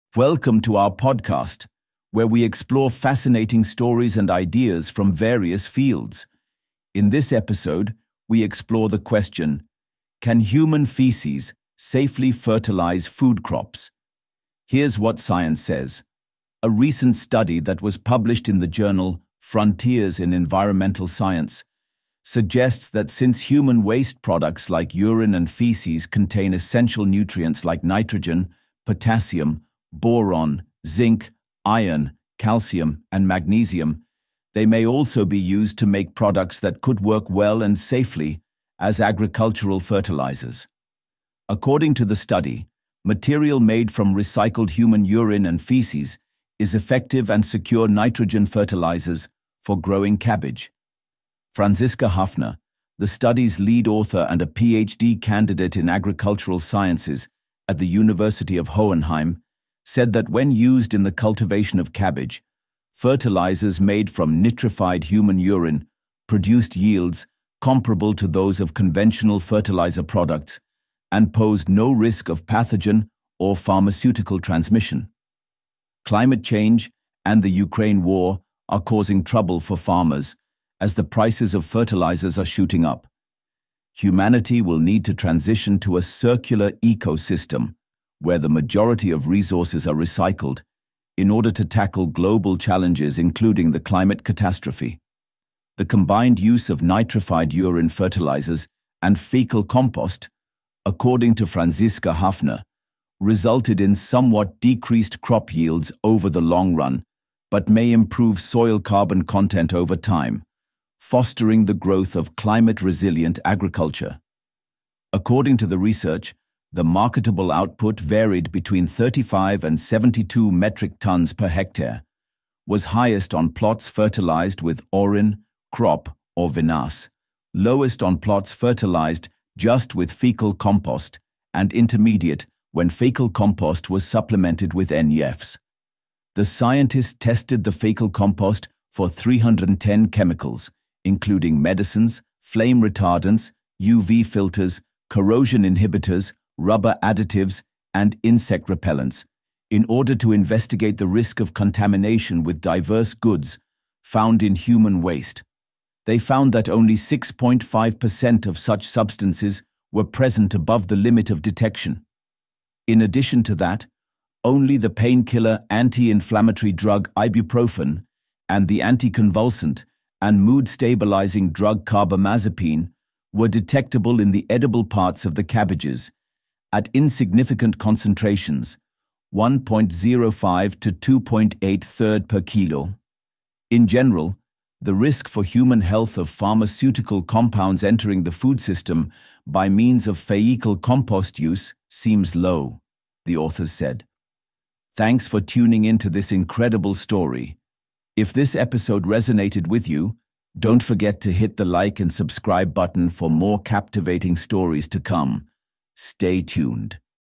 Human Waste as Fertiliser? New Study Says Yes! | WION Podcast
A new study published in Frontiers in Environmental Science suggests that human urine and faeces can be safely recycled into agricultural fertilisers. Researchers found that crops like cabbage grew just as well with these fertilisers as with conventional ones. The study also found minimal risk of pharmaceutical contamination in the edible parts of the plants. With rising fertiliser costs due to climate change and global conflicts, this sustainable solution could help build a circular, climate-resilient farming system. Could this be the future of eco-friendly agriculture?